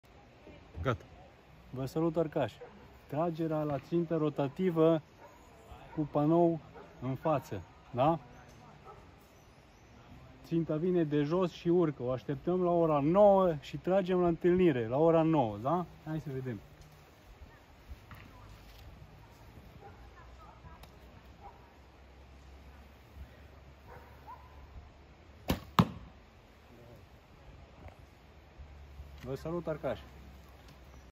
Ținte tehnice #2. Morișcar - rotatie jos in sus !
Filmat : Grindeanu Marius
Arc : Bodnick Recurve Nokoni, 40#,28”
Săgeti: lemn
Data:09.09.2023
Băile Tușnad - Finala CN Arc Istoric (F.R.A.I.)
Video scurt, pentru că am tras in timpul concursului, la sfărșitul tragerii de grupă la aceasta țintă. Urma să apară cealaltă grupă la acessta țintă.
 La aceasta morișca in fața ei după cum se vede este un panou care maschează palele rotative astfel încât să ai doar cîteva secunde in vizor ținta ( disc pe o spiță/pală ).
Trebuie să aștepti ținta într-o poziție convenabilă ( eu am așteptat-o la “ora 9” ) și să tragi la  “întâlnire “.
In concurs am tras 3 săgeți din 3 in morișcă ! 30 p.
In finală am tras 2 săgeți din 3 in morișcă ! 20 p 🤦‍♂️